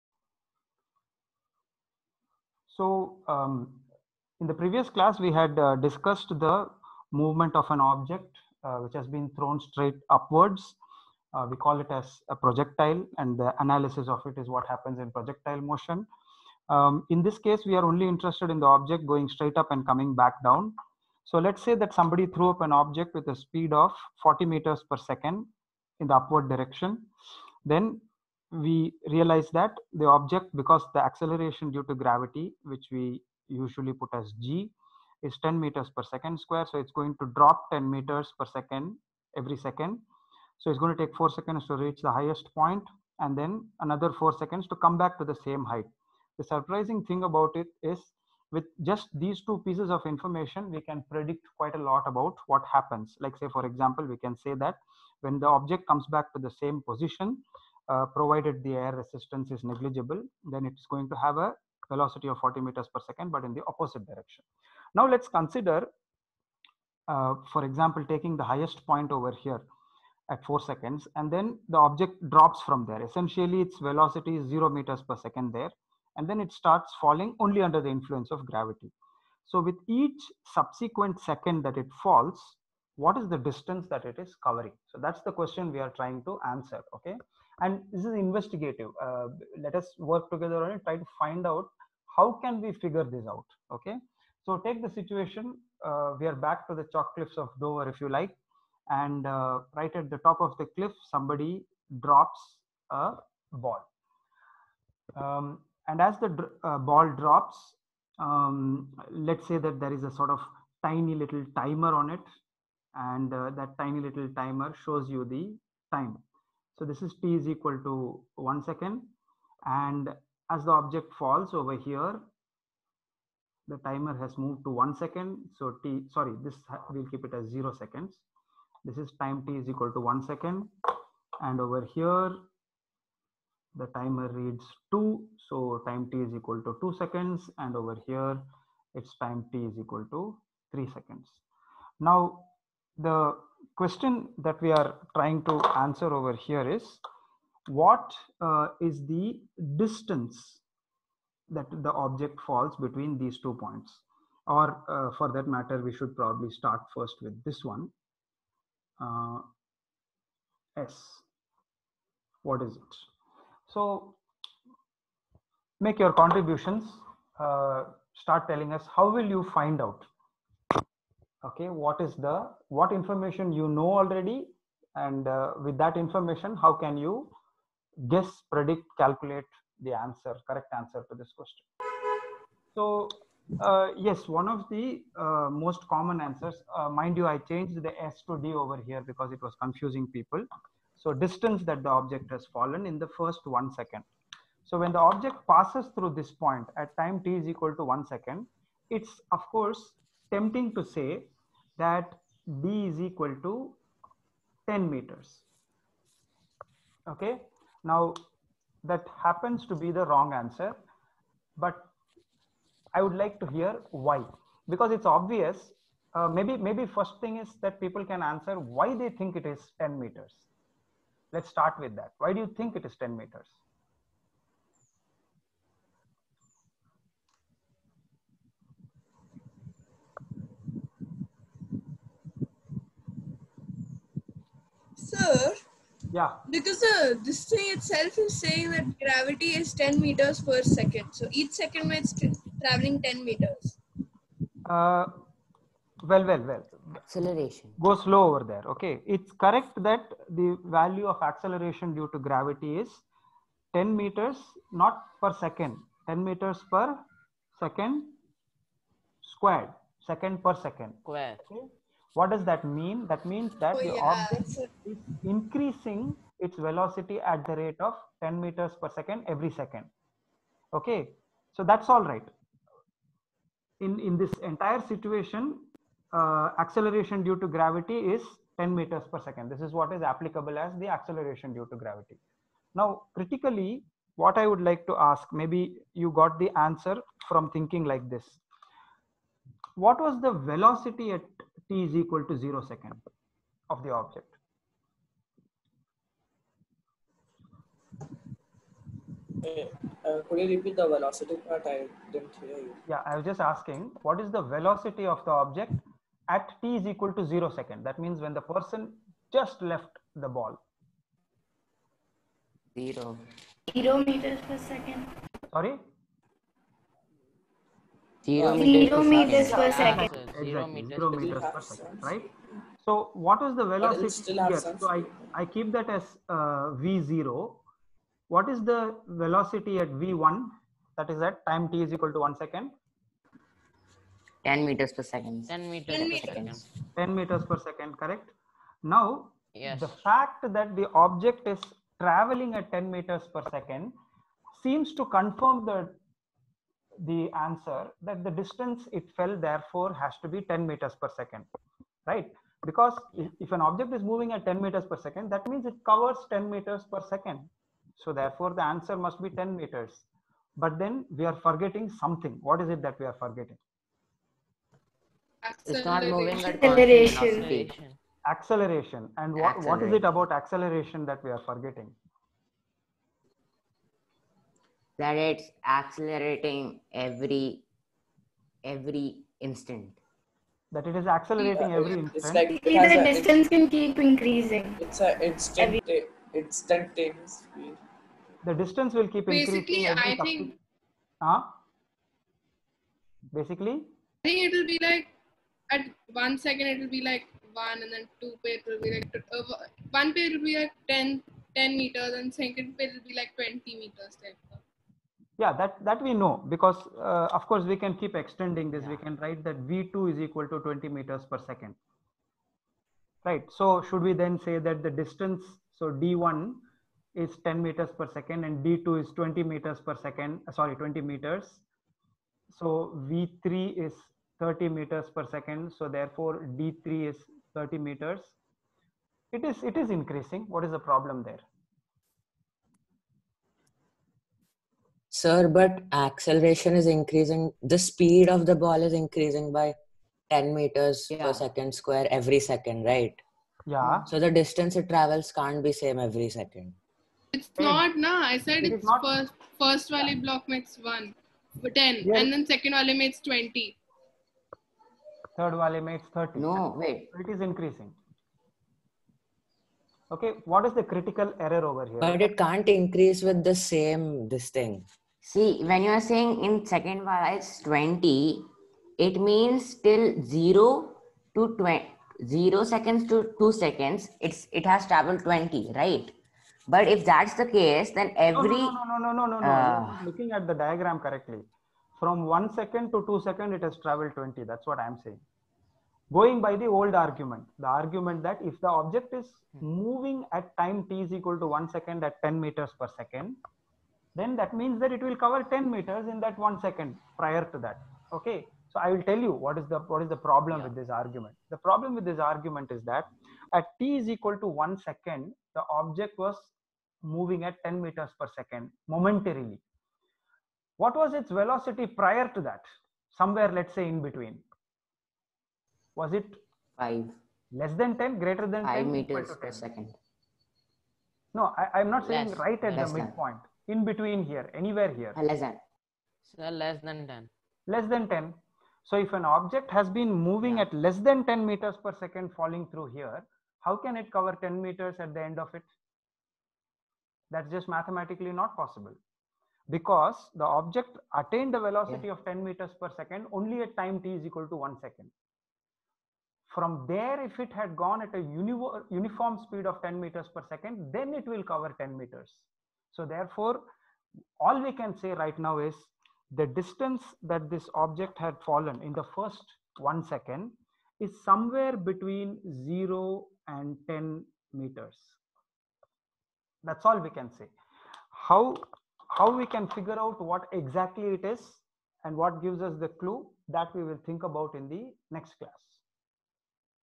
LM07: How Far Do Objects Fall? (Framing The Question)
When objects fall under the influence of gravity (freefall) how far do the objects fall in equal intervals of time? We know that the acceleration due to gravity has an approximate value of 10 m/s². If the object is "dropped" and it gains a velocity of 10 m/s every second, should it cover 10 m in one second? What is the problem with this approximation?